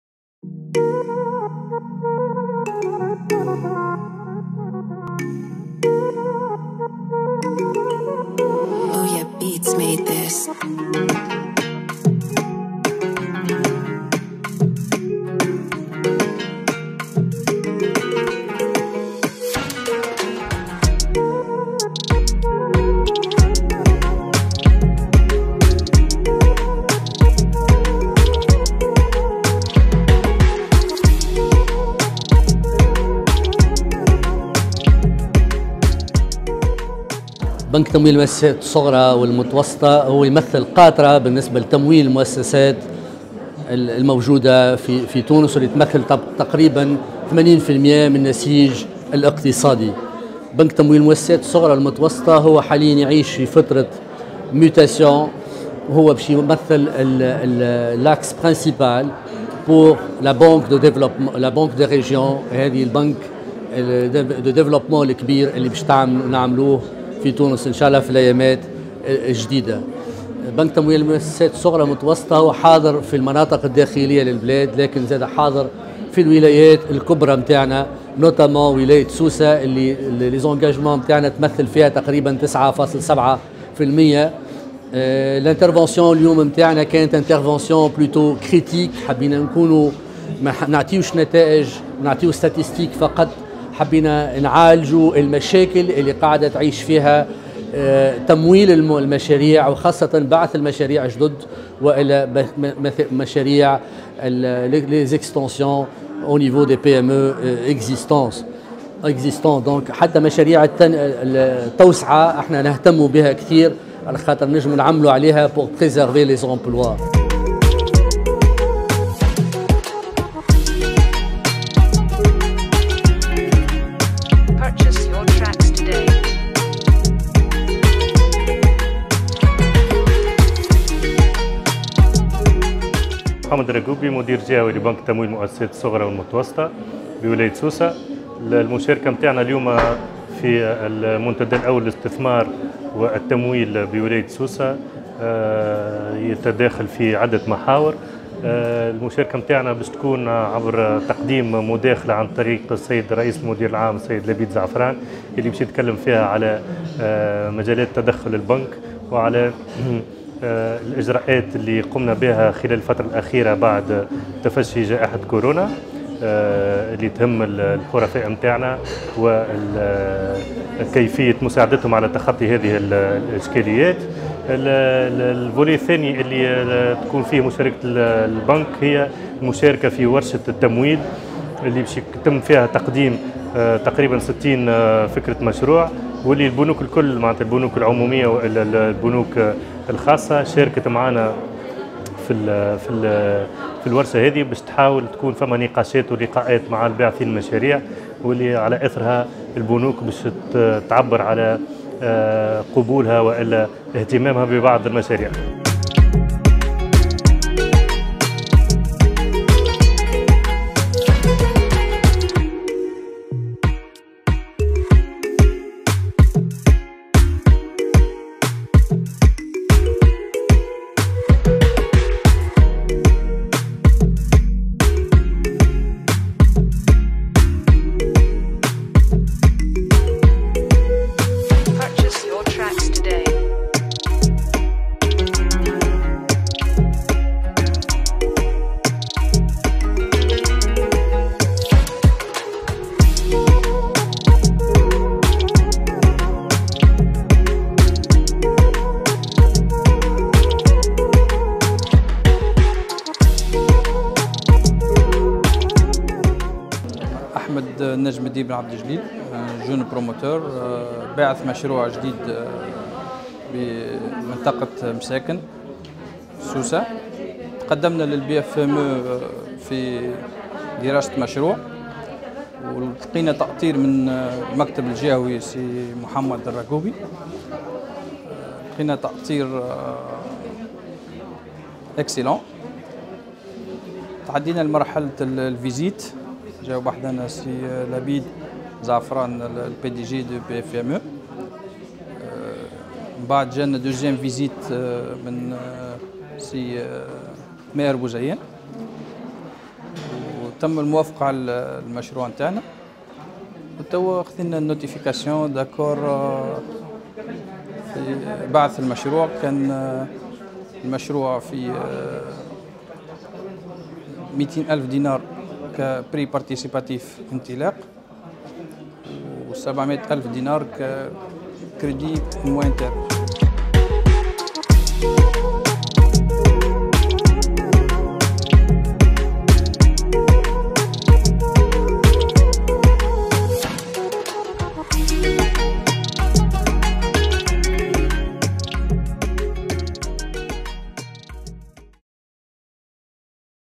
Premier Forum de Sousse pour L’investissement et Le Financement le 28 Sep 2020 à Sousse
1er Forum de Sousse pour L’investissement et Le Financement : Investir en Période de Crise le 28 Septembre 2020 à l'hôtel THE PEARL , Sousse